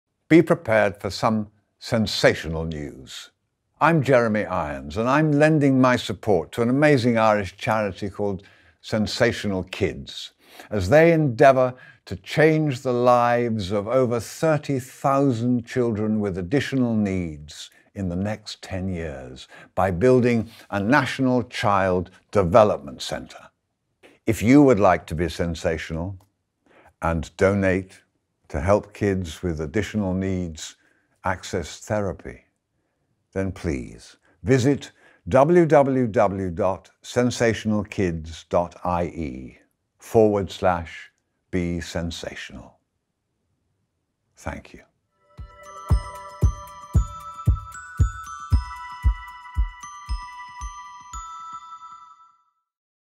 Be Sensational! - A special video message from award winning actor Jeremy Irons
A special video message from award winning actor Jeremy Irons about Sensational Kids and their visionary initiative to build a National Child Development Centre in Kildare, Ireland which will make a significant difference to the lives of children and adolescents with additional needs.

You too can you play your part in this story by donating